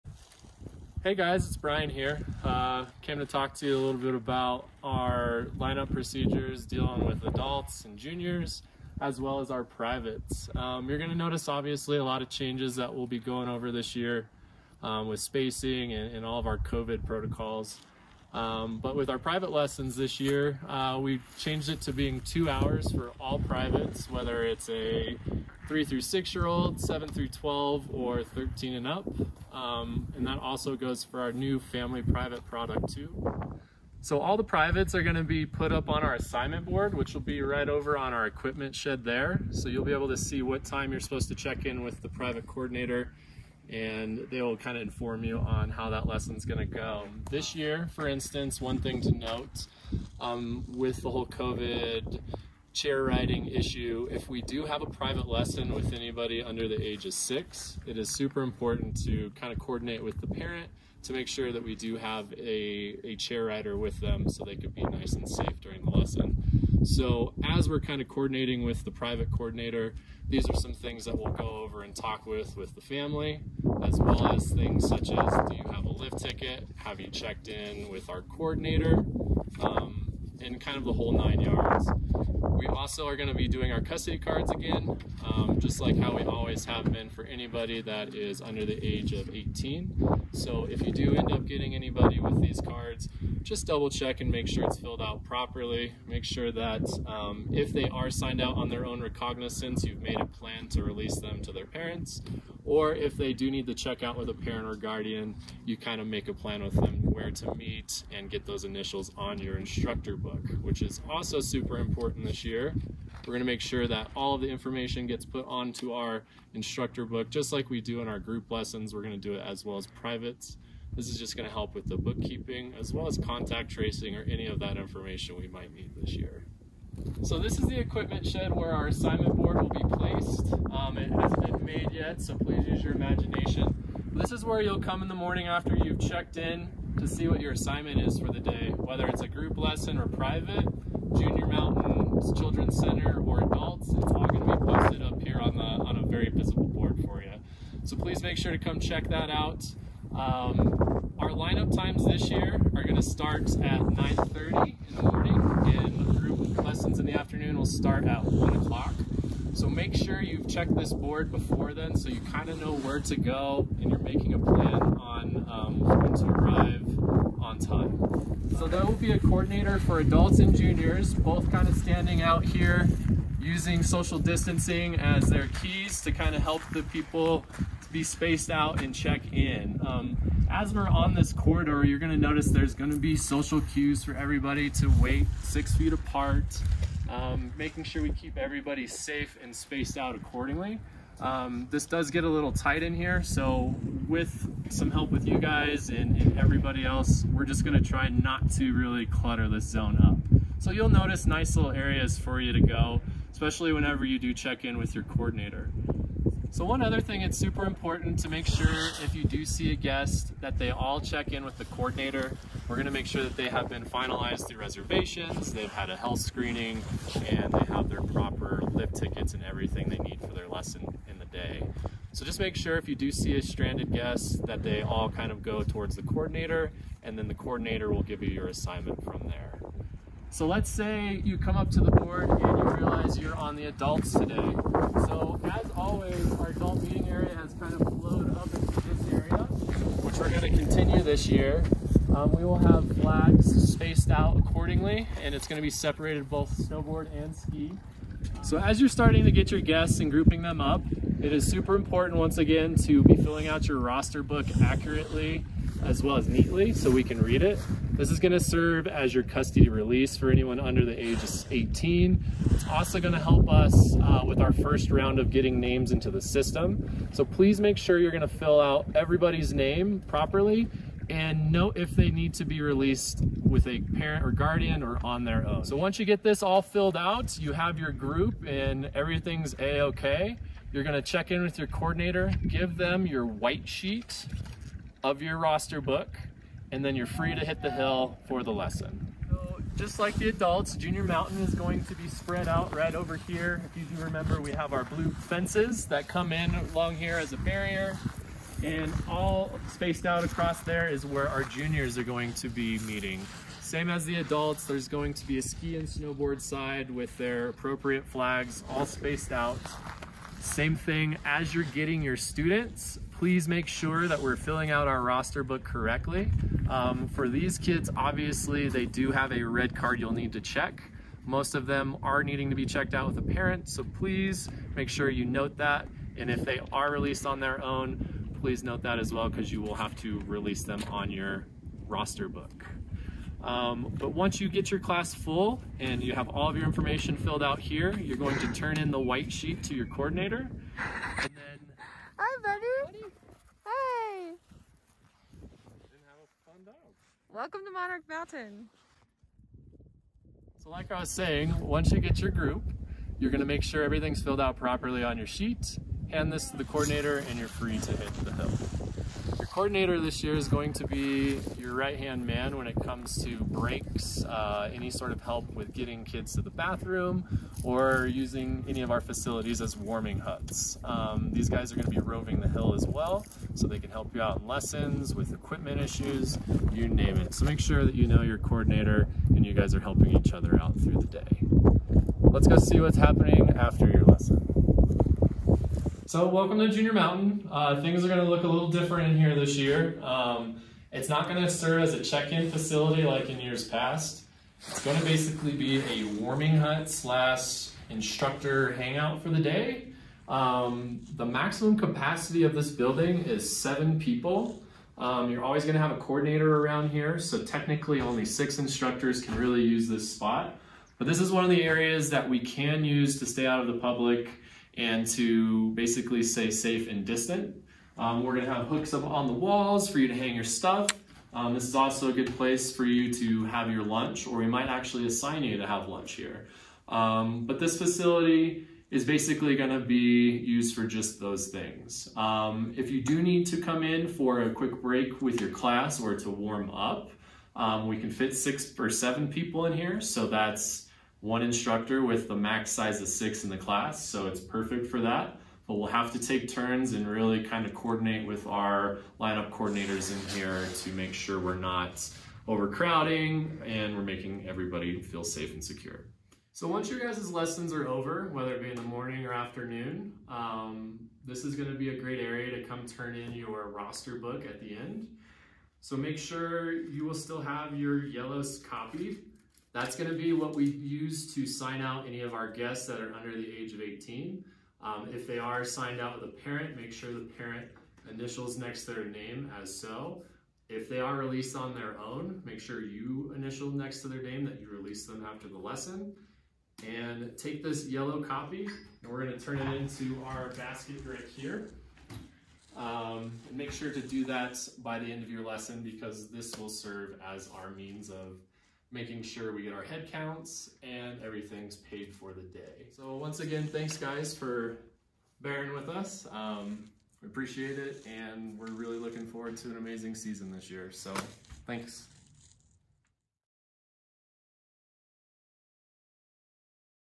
Group and Private Lesson line-ups
Bryan talks about the Group and Private lesson meeting areas and protocals